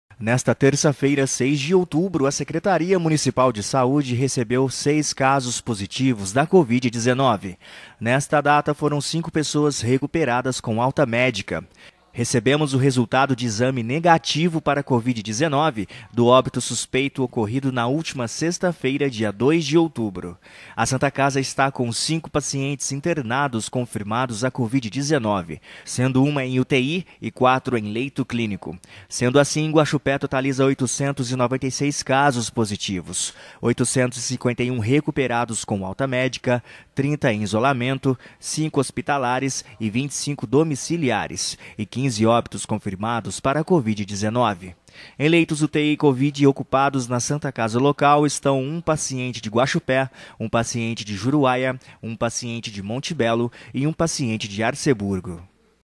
GUAXUPÉ REGISTRA CINCO PESSOAS RECUPERADAS E SEIS CASOS POSITIVOS DA COVID-19
Nesta terça-feira, 06 de outubro, a Secretaria Municipal de Saúde recebeu 06 casos POSITIVOS da COVID-19.

Na data de hoje tivemos 05 pessoas recuperadas/em alta médica. Recebemos o resultado de exame NEGATIVO para COVID-19 do óbito suspeito ocorrido na última sexta-feira, dia 02 de outubro.

A Santa Casa está com 05 pacientes internados confirmados a Covid-19, sendo 01 em UTI e 04 em leito clínico.

Sendo assim, Guaxupé totaliza 896 casos positivos:
- 851 recuperados (alta médica)
- 30 em isolamento (05 hospitalares e 25 domiciliares)
- 15 óbitos confirmados COVID-19

 LEITOS UTI COVID-19 NA SANTA CASA LOCAL:*
*Divulgação solicitada pela Santa Casa

TOTAL DE LEITOS UTI COVID OCUPADOS: 04
- 01 paciente de Guaxupé
- 01 paciente de Juruaia
- 01 paciente de Monte Belo
- 01 paciente de Arceburgo